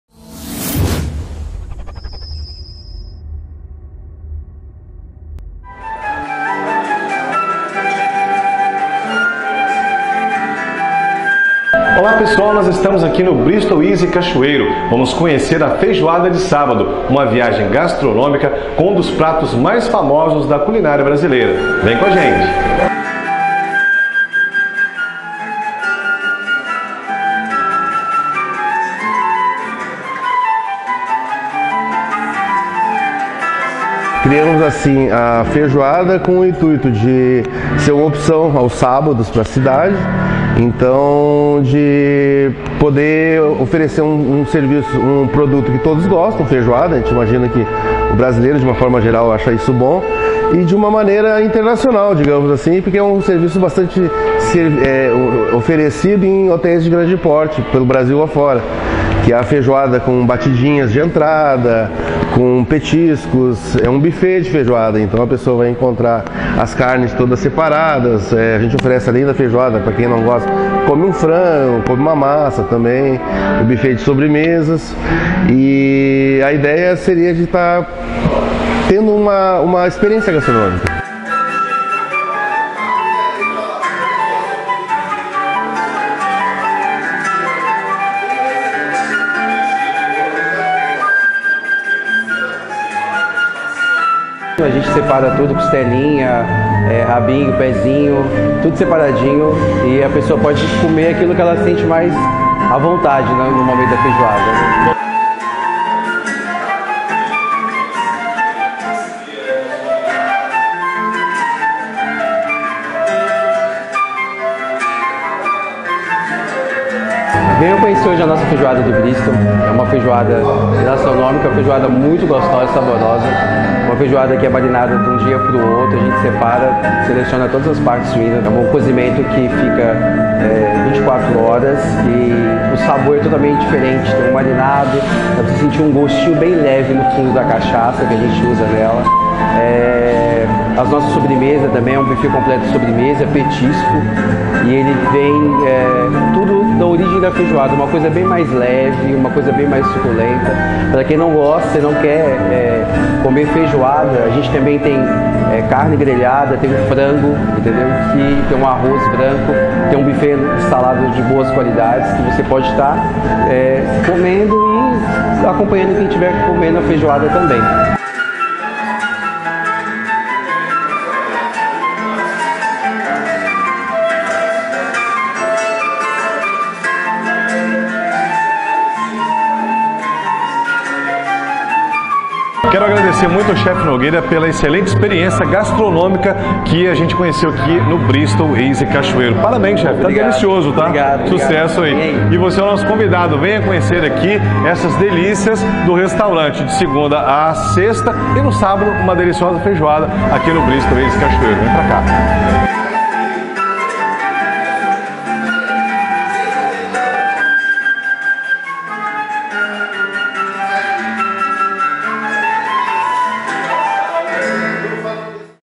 FEIJOADA ESPECIAL - invadimos a feijuca de um dos hotéis mais concorridos do ES
Visite e veja várias fotos.